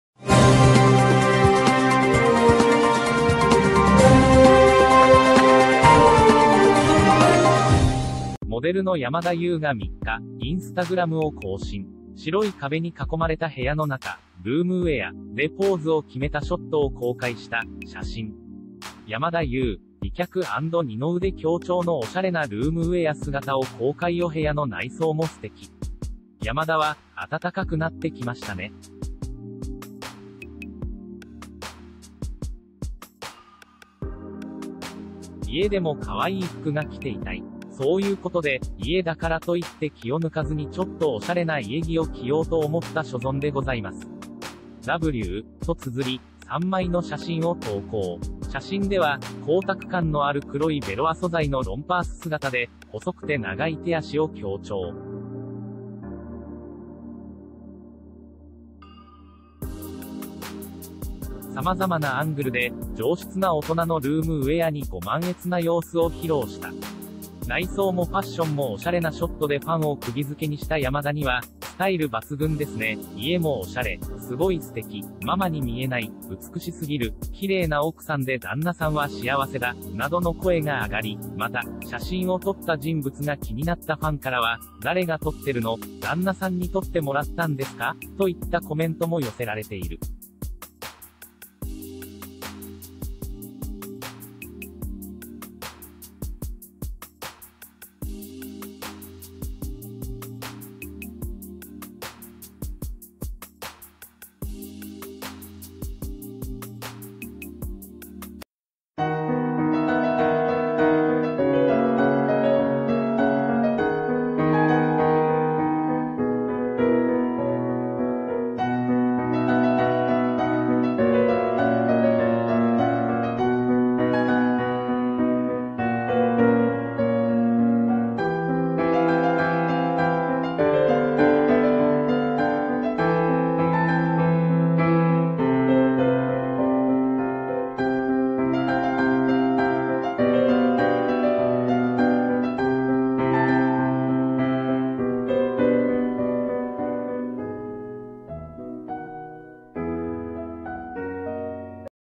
山田優の美脚強調“おうちショット”に羨望の眼差し…「誰に撮ってもらったの？」
白い壁に囲まれた部屋の中、“ルームウエア”でポーズをきめたショットを公開した。【写真】山田優、美脚&二の腕強調のオシャレなルームウエア姿を公開　お部屋の内装も素敵!　山田は「暖かくなってきましたね。。。家でも可愛い服が着ていたい。。。そういうことで、、、家だからといって気を抜かずにちょっとお洒落な家着を着ようと思った所存でございます。w」とつづり、3枚の写真を投稿。  　写真では、光沢感のある黒いベロア素材のロンパース姿で、細くて長い手足を強調。様々なアングルで、上質な大人のルームウエアにご満悦な様子を披露した。  　内装もファッションもオシャレなショットでファンを釘付けにした山田には、「スタイル抜群ですね」「家もおしゃれ！」「すごい素敵」「ママに見えない」「美しすぎる」「綺麗な奥さんで旦那さんは幸せだ」などの声があがり、また、写真を撮った人物が気になったファンからは、「誰が撮ってるの？」「旦那さんに撮ってもらったんですか？」といったコメントも寄せられている。